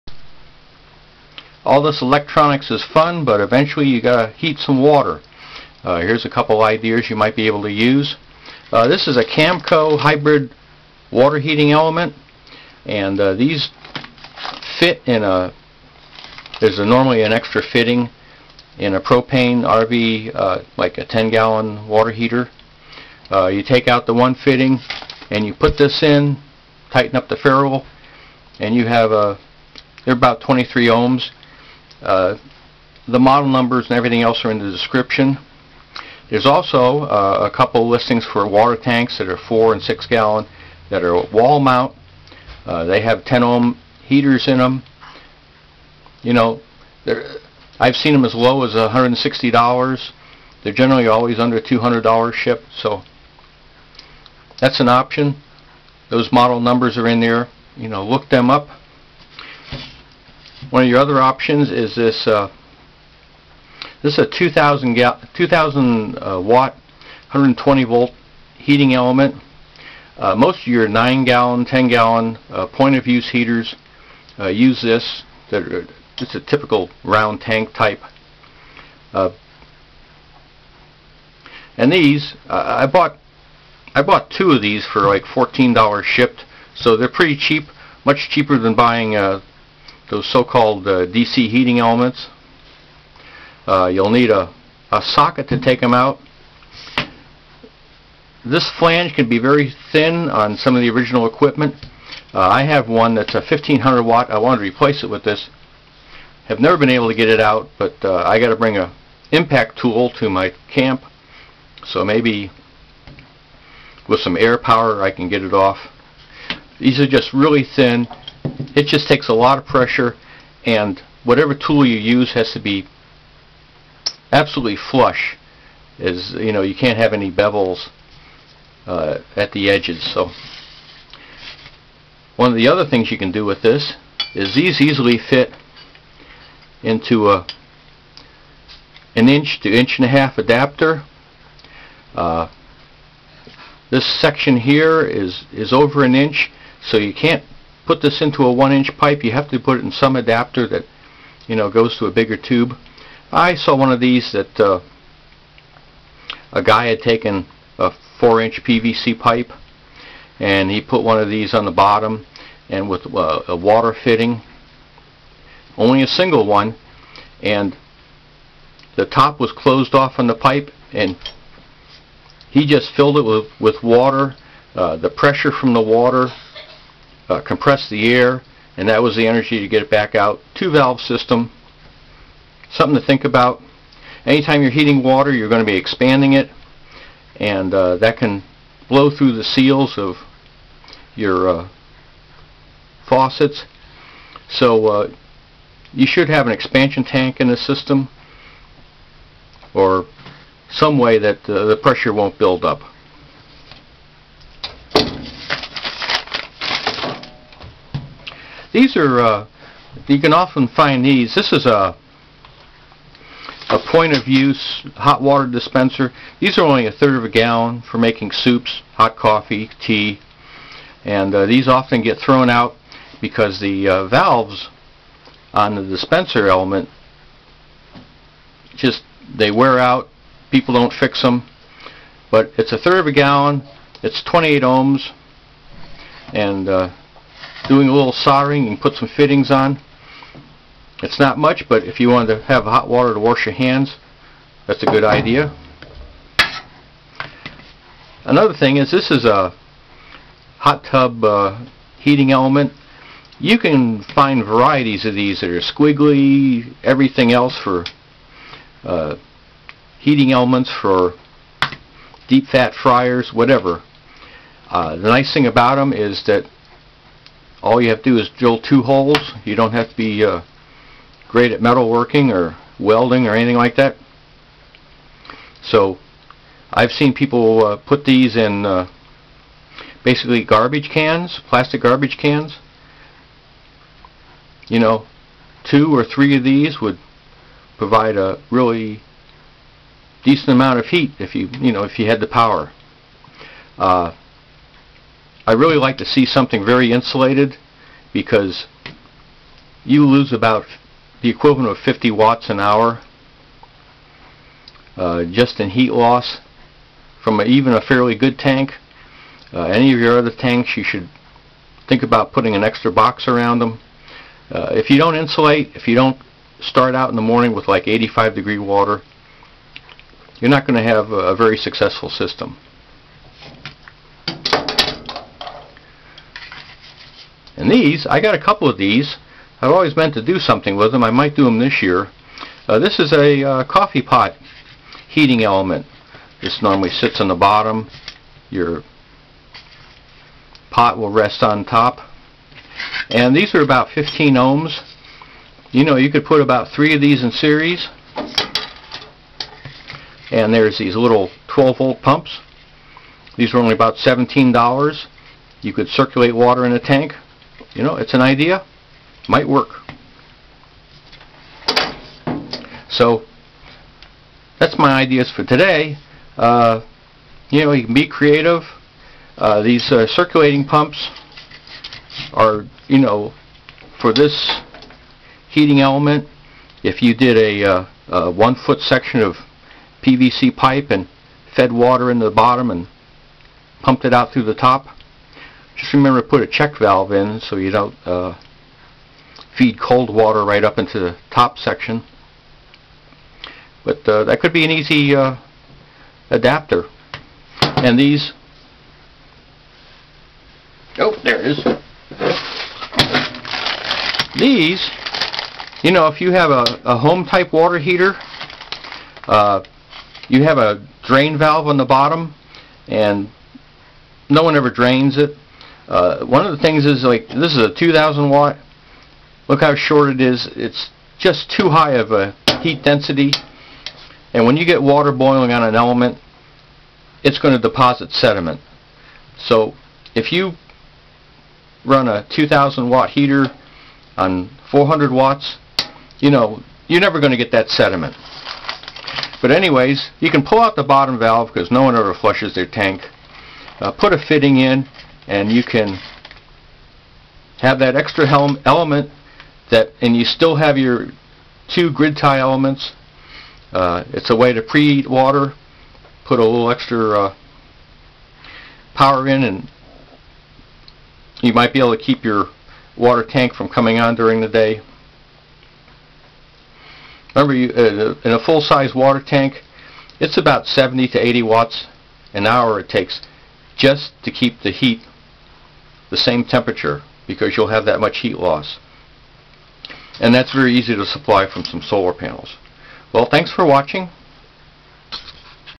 Water Heater Element Options, including Dumpster Coffee Makers
Camp water heater from dumpster coffee makers and other heating elements. You don't need to buy expensive DC heater elements. Propane electric hybrid elements. Wall mount 4-6 gallon water heater W/temp control like ECCOTEMP EM-4.0 and WAIWELA WM-6.0 as examples under $200.

The Camco 11774 is an additional thin heating element that can be placed into a tank with any port.
It is 8 inches long and is trated 635W at 120V. Do not confuse this with 11674 which is shorter at 6 1/2 inches and 425W. Both elements are less than $20. These come with a threaded brass adaptor with ferule seal. Installation can be seen by watching the complete kit 11673 hybrid heater video. A threaded collar with a sacrificial anode is also available.

1774 625W 120V thin 8 inch element
Representative watts at operating volts.
625/120 = 5.2A  120/5.2 = 23 ohms

50V/23 = 2.2A  50V * 2.2   = 110W

60V/23 = 2.6A  60V * 2.6   = 156W

70V/23 = 3.0A  70V * 3.0   = 210W

80V/23 = 3.5A  80V * 3.5   = 280W

90V/23 = 3.9A  90V * 3.9   = 352W

100V/23 = 4.3A  100V * 4.3 = 430W

02202 2,000W 120V element (standard water heater element)
2000/120 = 16.6A  120/16.6 = 7.3 ohms

30V/7.3 = 4.1A  30V * 4.1   = 120W

50V/7.3 = 6.8A  50V * 6.8   = 340W

60V/7.3 = 8.2A  60V * 8.2   = 492W

70V/7.3 = 9.6A  70V * 9.6   = 670W

80V/7.3 = 10.9A 80V * 10.9  = 875W

90V/7.3 = 12.3A 90V * 12.3  = 1100W